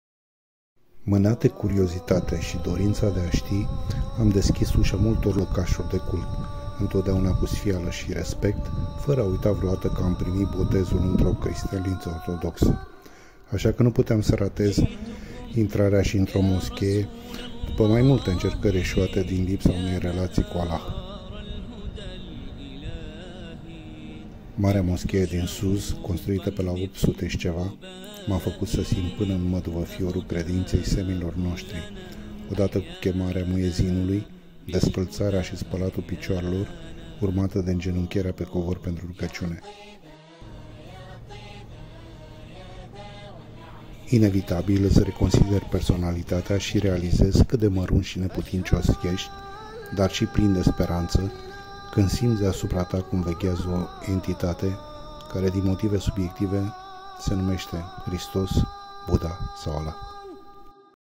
Marea moschee din Sousse, Tunisia
Un lucru neobișnuit la această moschee este acela ca nu are minarete. În Tunisia, marile moschei sunt în general situate în centrul medinelor. Absența unui minaret este, de asemenea, neobișnuită. Turnul din ribatul vecin este folosit pentru a chema credincioșii la rugăciune. Clădirea a fost remodelată și mărită în secolele al X-lea și al XVII-lea rezultand un aspect masiv și auster.